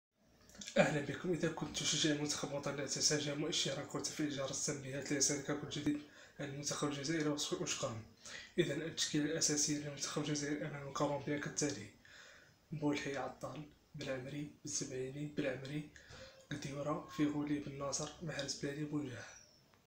التشكيلة الأساسية للمنتخب الجزائري أمام كولومبيا
يلعب جمال_بلماضي أمام كولومبيا بالتشكيلة التالية
مبولحي
عطال ماندي بلعمري بن سبعيني
بن ناصر فغولي قديورة
رياض_محرز بلايلي بونجاح
و القنوات الناقلة لمباراة الجزائر كولومبيا هي الأرضية الجزائرية